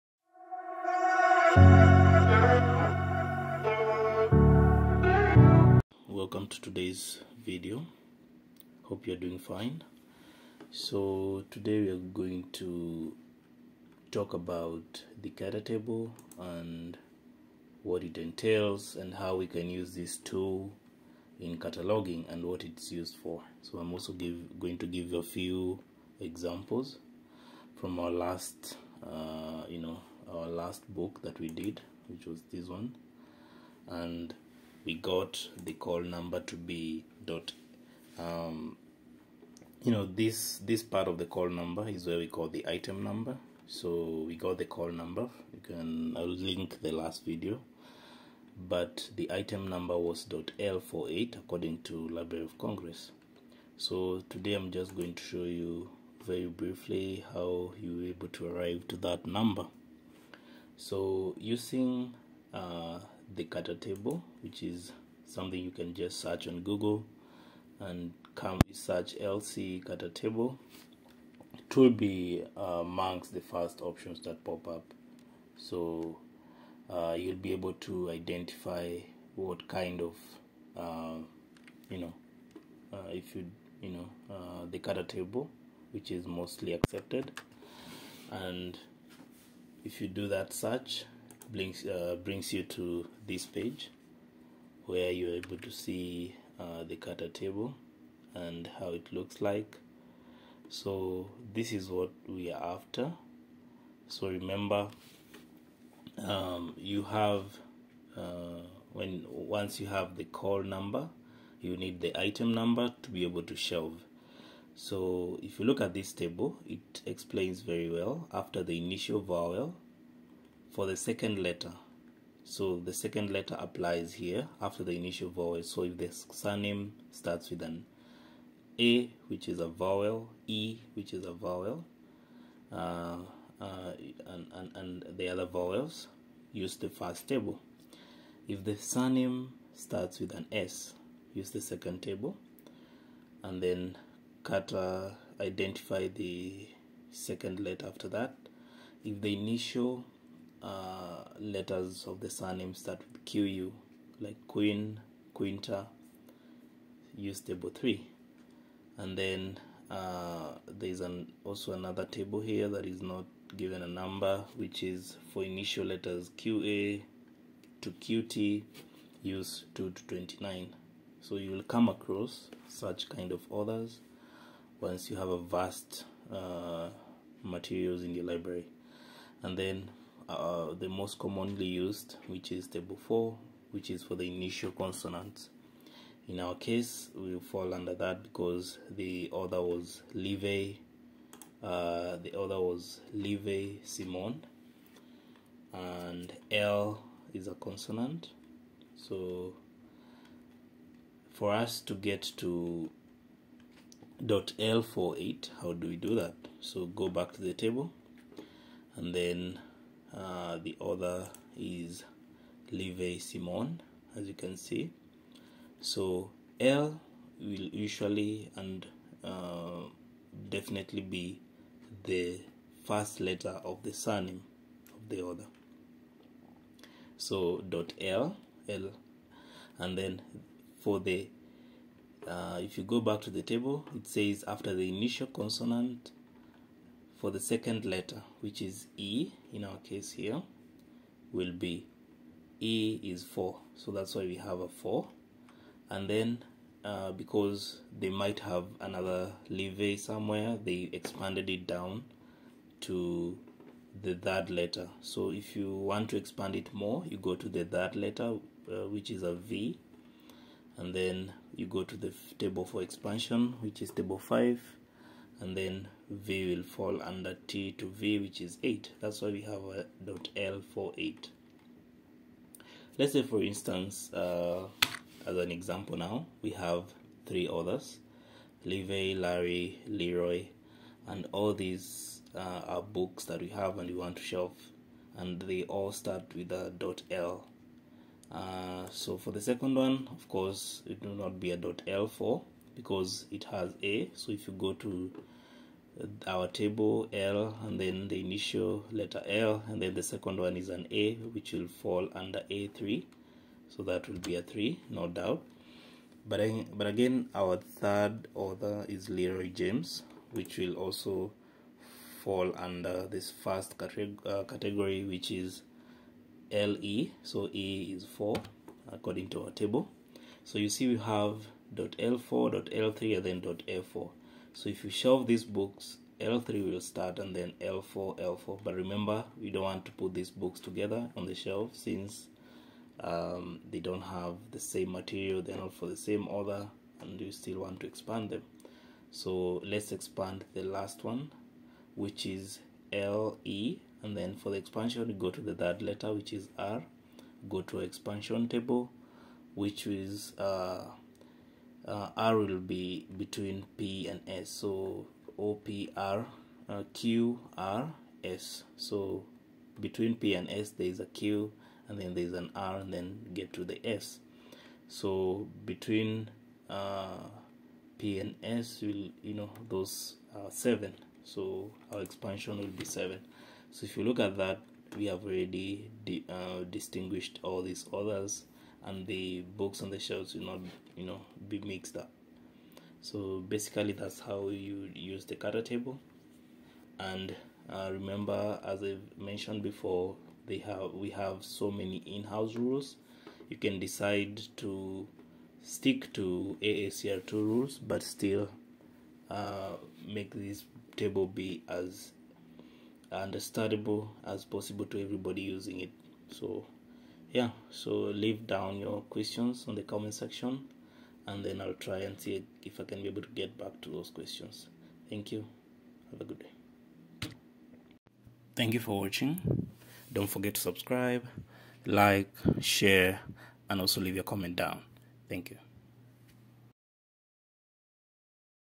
How to use a cutter table in LC (3 examples)
This is how we got the Item number (.L48) using the LC Cutter table.